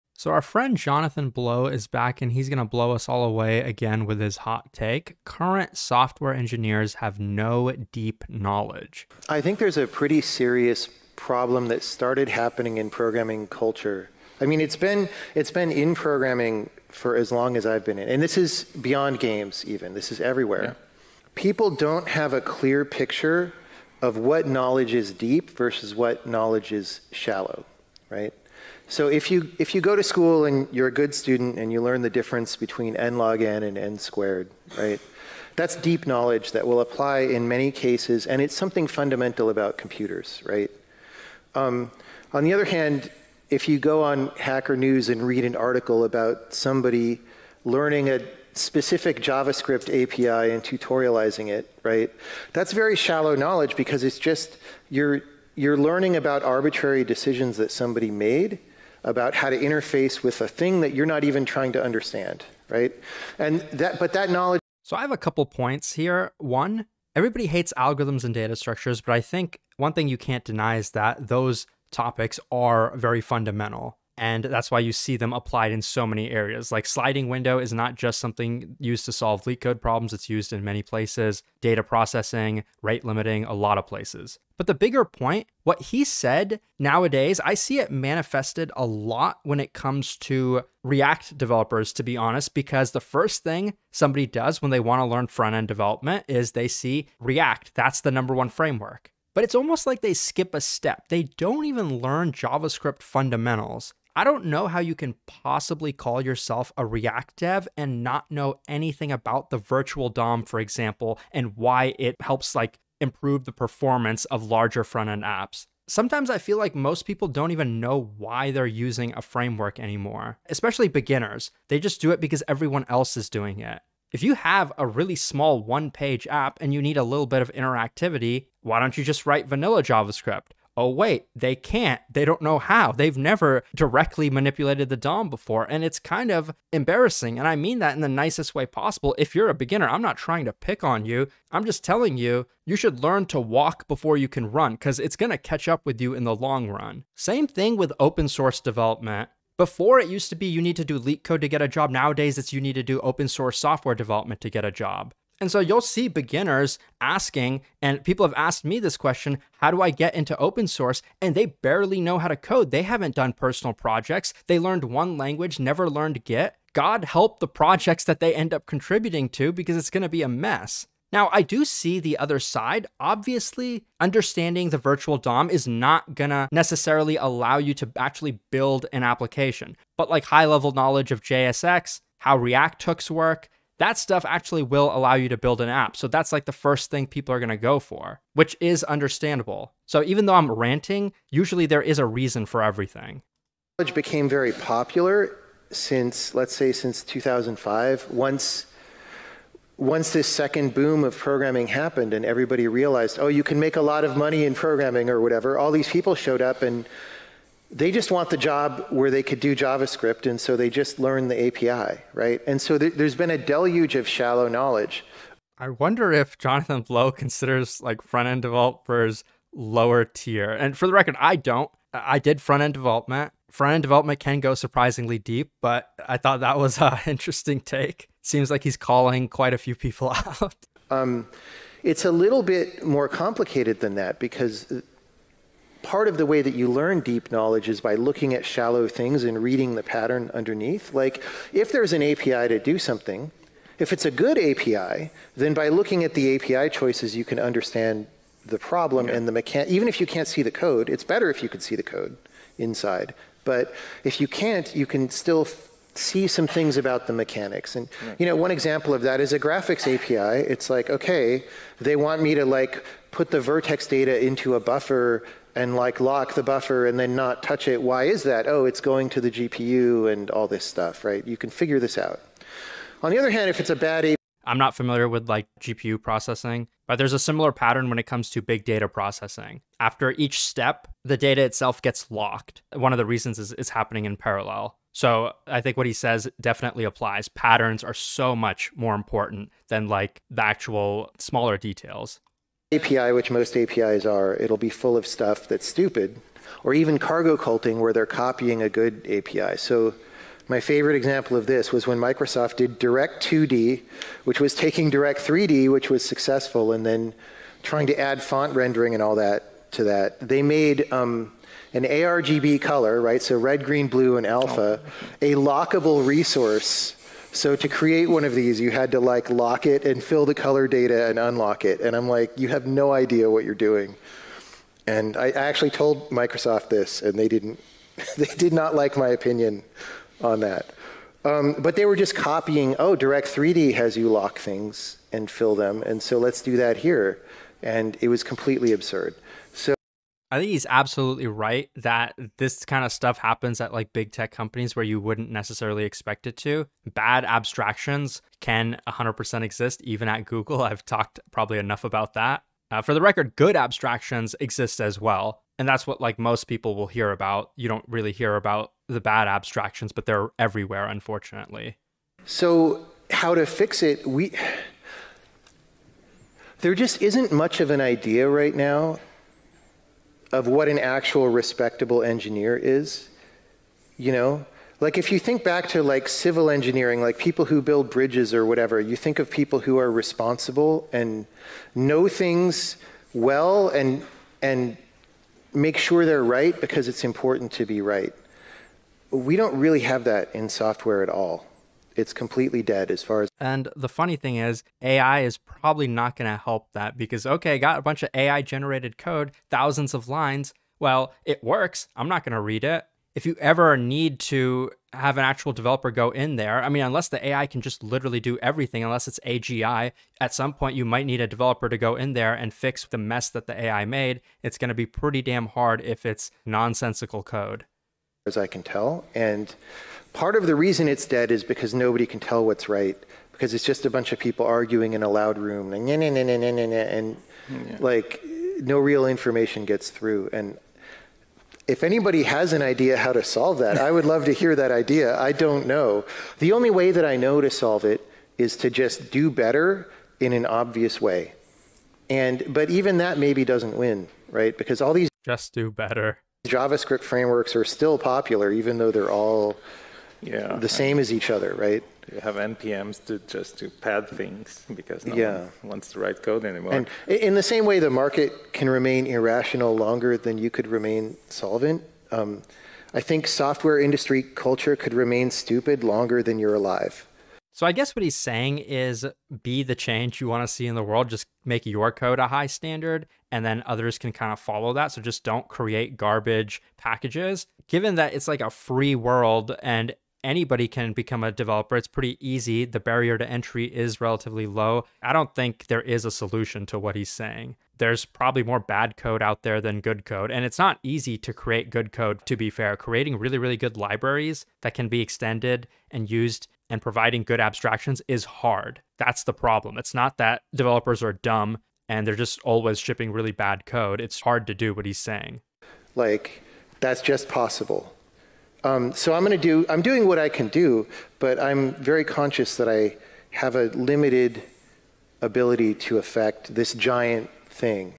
Programmers in 2024 have no Deep Knowledge
Jonathan Blow has a point, but deep knowledge isn't required much these days. Which makes programming boring imo.